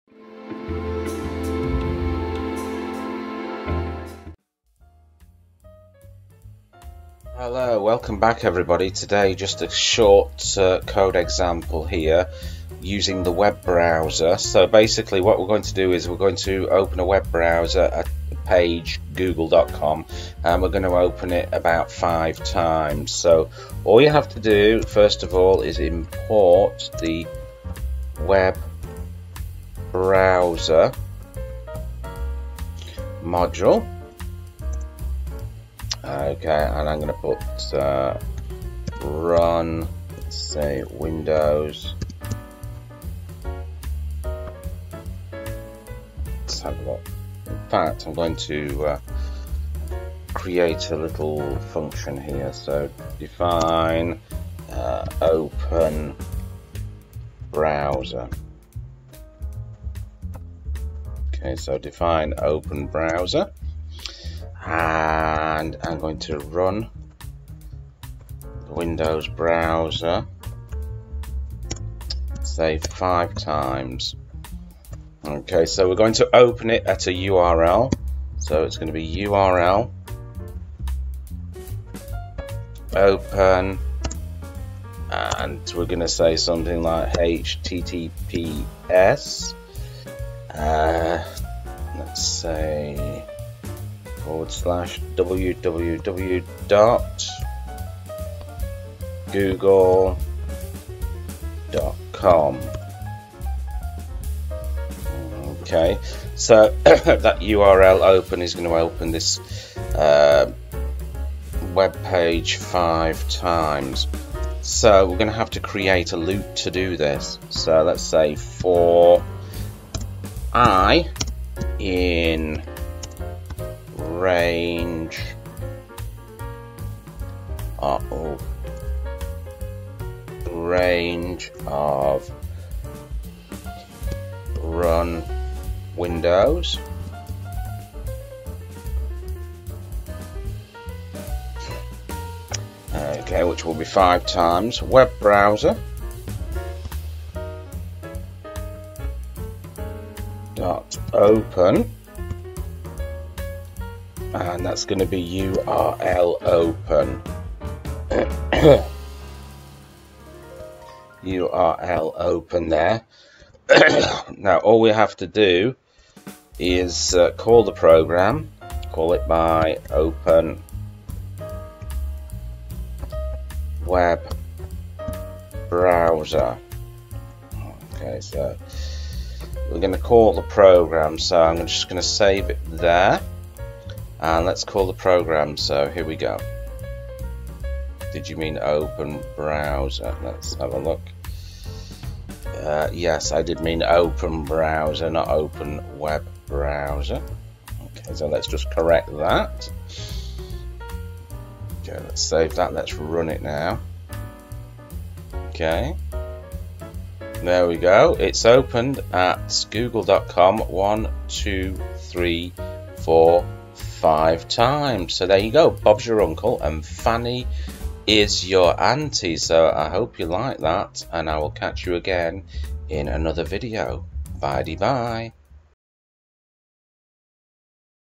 Python Beginners- Open Webbrowser 2023
Open And Run A Wb Browser Using The Python Webbrowser Module. Beginners Tutorial 2023.